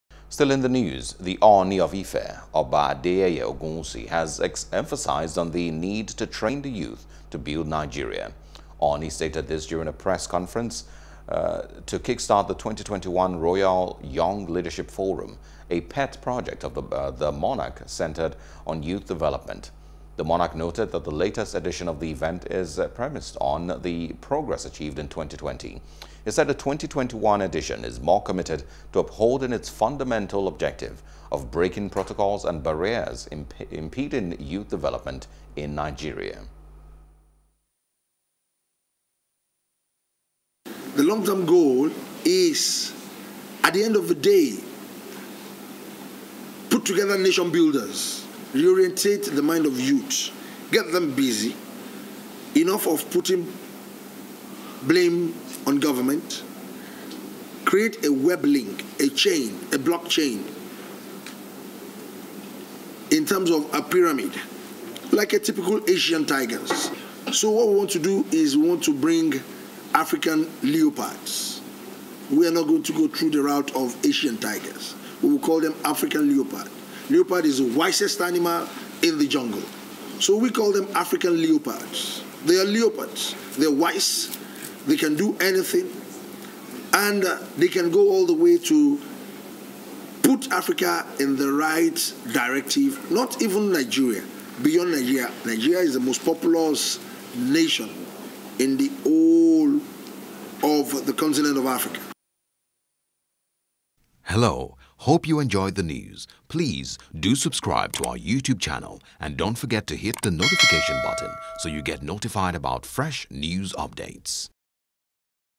Oba Adeyeye Ogunwusi: Ooni Of Ife Restates Commitment To Training Youth For Nation Building | NEWS
The Ooni of Ife, Oba Adeyeye Ogunwusi has emphasized on the need to train the youth to build Nigeria.

Ooni stated this during a press conference to kick-start the 2021 Royal Young Leadership Forum, a pet project of the monarch centred on youth development.